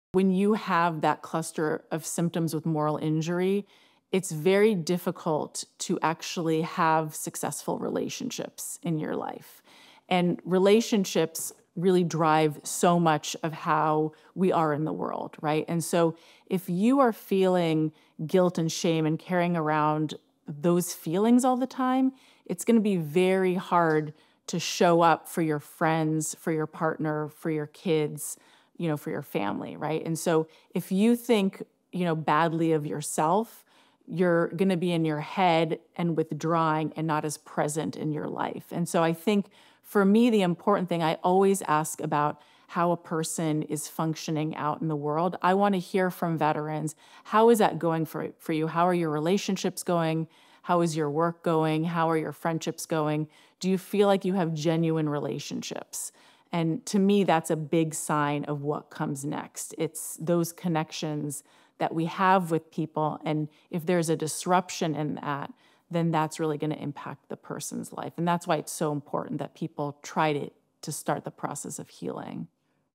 If you are feeling guilt and shame, it's going to be very hard to show up for your friends.
Dr. Shira Maguen knows how important it is to have successful relationships, and guilt and shame from moral injury can really get in the way of those relationships. Learn about PTSD and moral injury from Dr. Shira Maguen — and Veterans who've been there